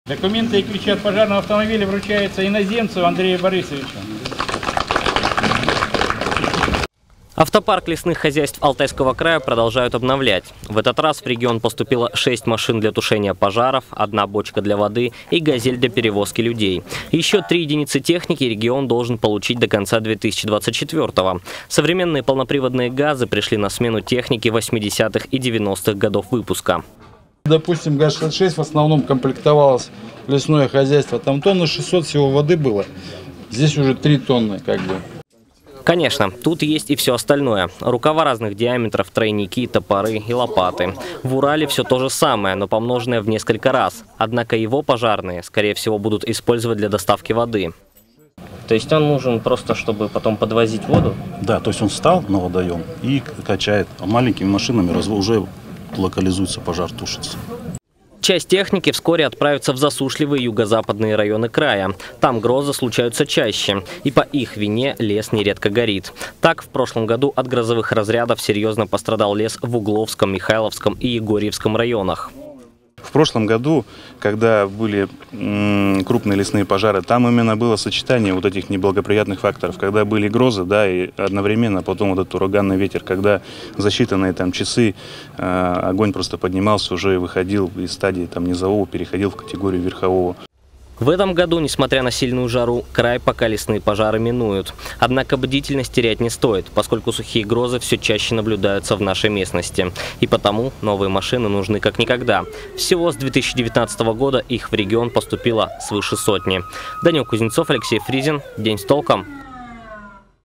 Лесные хозяйства Алтайского края получили новую пожарную технику
Число сухих гроз, по причине которых возникают лесные пожары, растет в Алтайском крае, утверждают специалисты. Разным мерам борьбы с возгораниями в последнее время они уделяют особое внимание. И уже несколько лет подряд стараются обновлять материальную базу лесных хозяйств. Сегодня сразу несколько районов края, где в прошлом году прошли крупные пожары, получили новую лесопожарную технику.  Она сменит  порядком изношенные машины 80-х и 90-х годов.